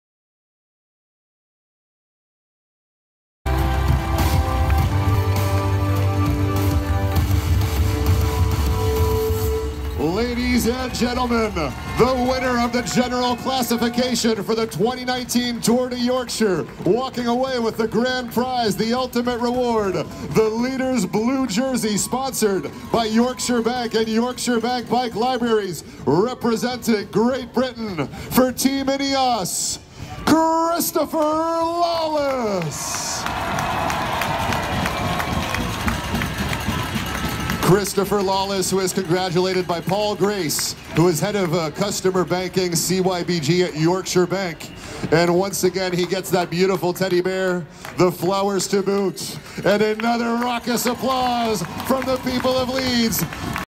Podium Tour de Yorkshire 2019 Winner Chris Lawless
Podium presentation for the 2019 winner of Le Tour de Yorkshire 2019, Chris Lawless of Team Ineos.